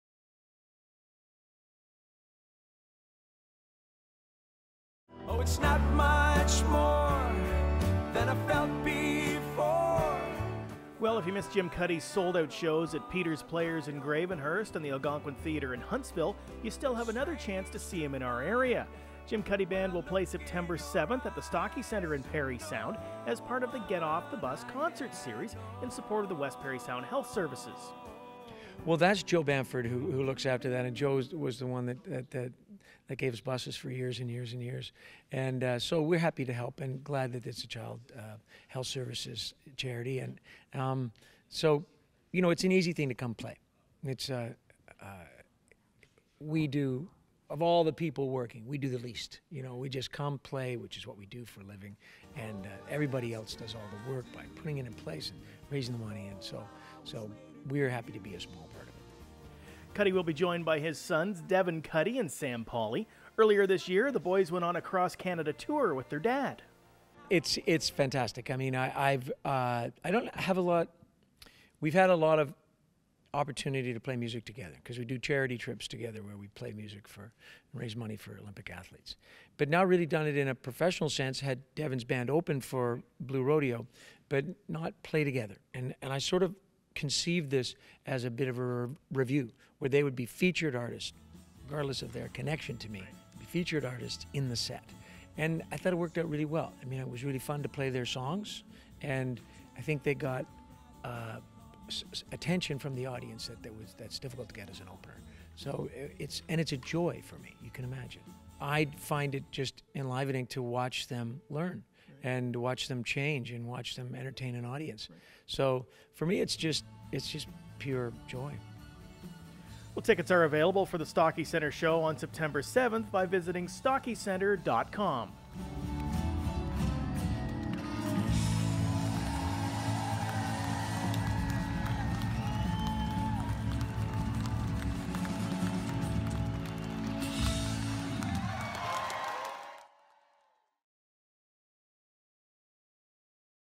Jim Cuddy Coming To The Stockey Centre
Jim Cuddy will perform September 7th at the Stockey Centre as part of the Get Off The Bus Concert series